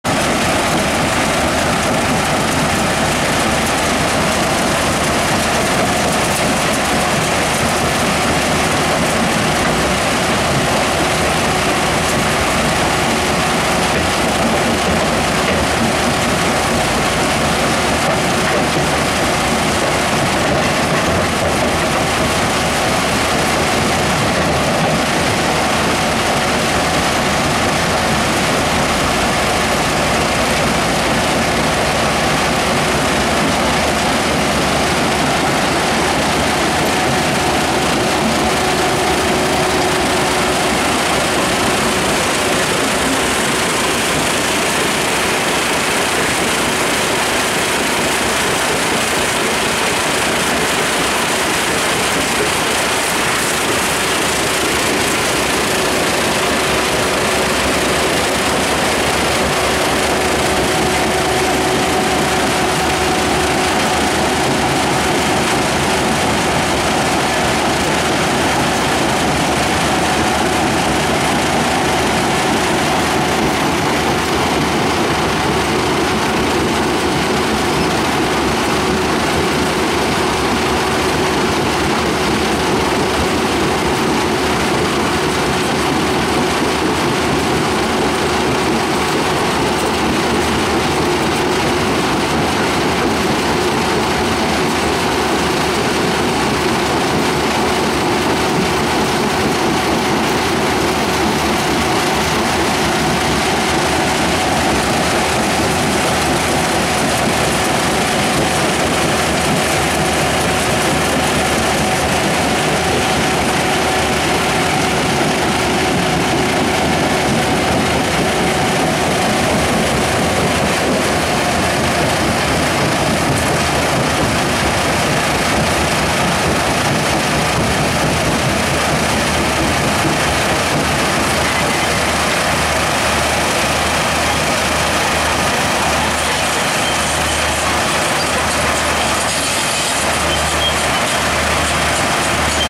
Getting a Rebel Crusher Ready to Ship
Testing the Rebel Crusher before shipping it out to the customer.

Crush Rock, Crush Limestone, Crush Granite, Crush Brick, Crush Block, Crush Concrete, Crush Asphalt, and so much more!

for brochures, photos, and more.